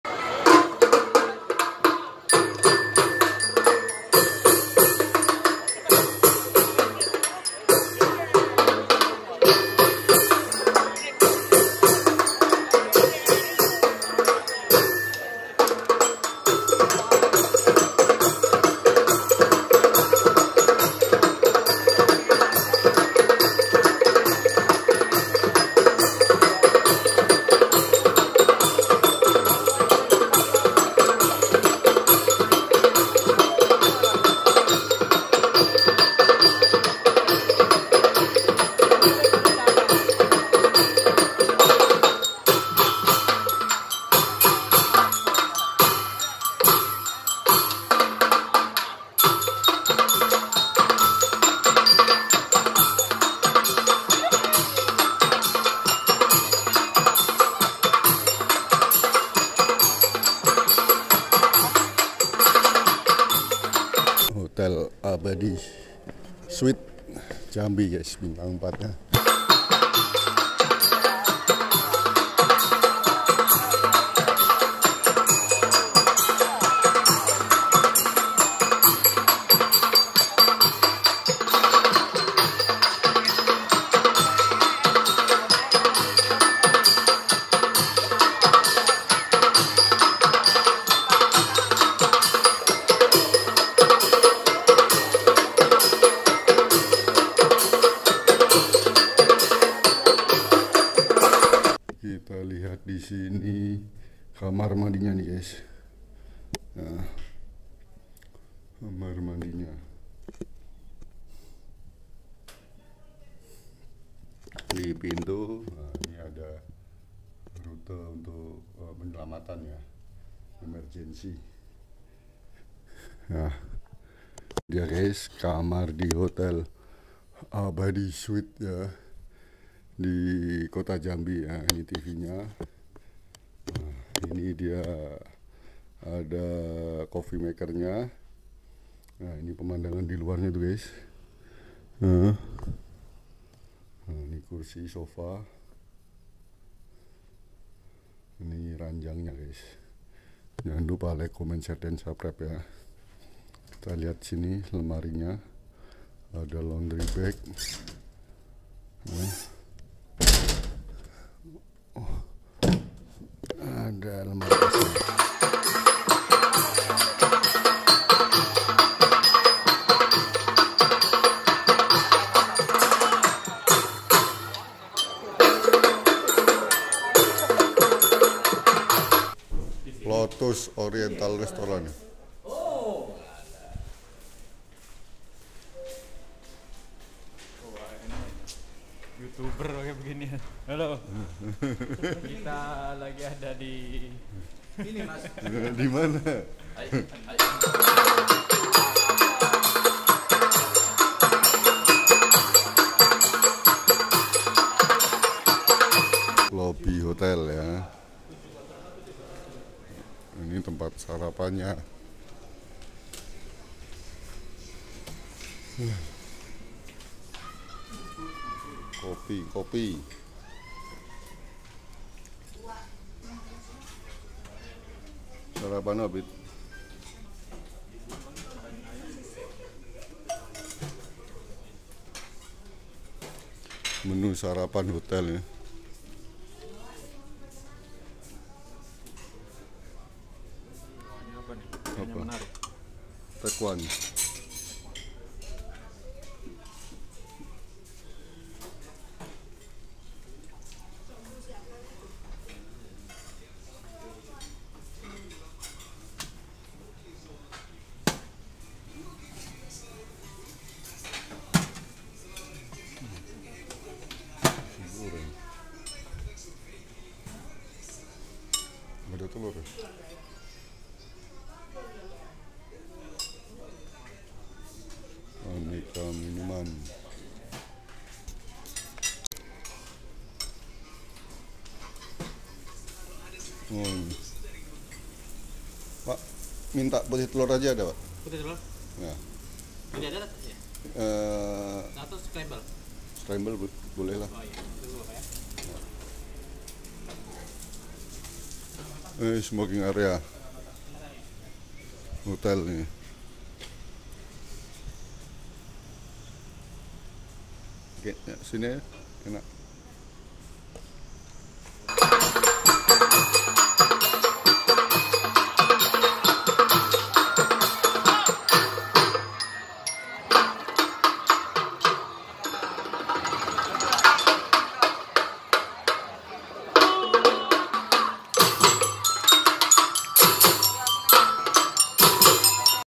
Hotel Abadi Suite Jambi
Hotel Abadi Suite adalah salah satu hotel berbintang empat yang ada di kota Jambi. Hotel ini di bangun dengan cukup mengah dan menjadi salah satu bangunan tertinggi di Kota Jambi. Hotel berada dipusat kota Jambi. Hotel memiliki kamar yang cukup luas.